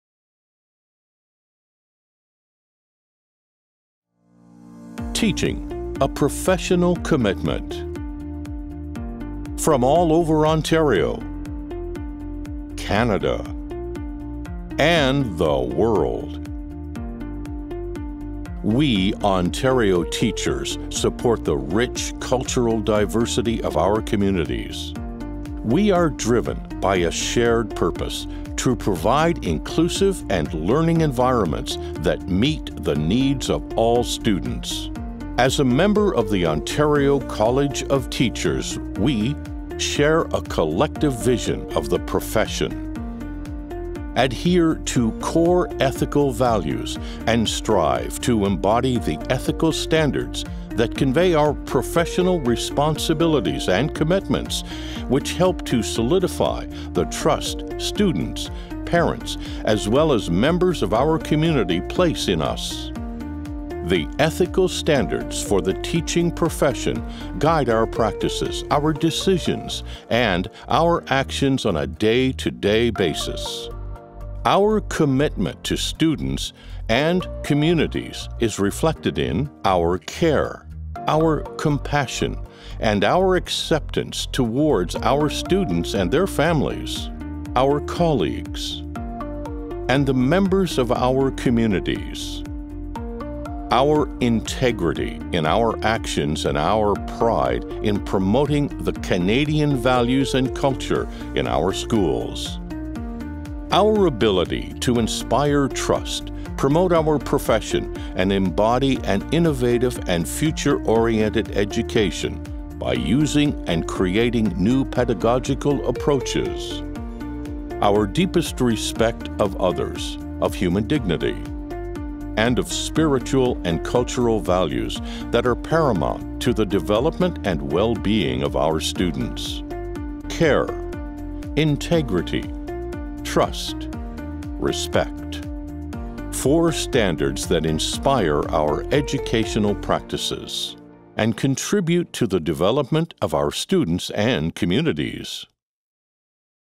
The Ethical Standards for the Teaching Profession
Our new vignettes visually reinforce the "Ethical Standards for the Teaching Profession" and the "Standards of Practice for the Teaching Profession." They can be used in initial teacher education, continuing teacher education, leadership development and within professional practice.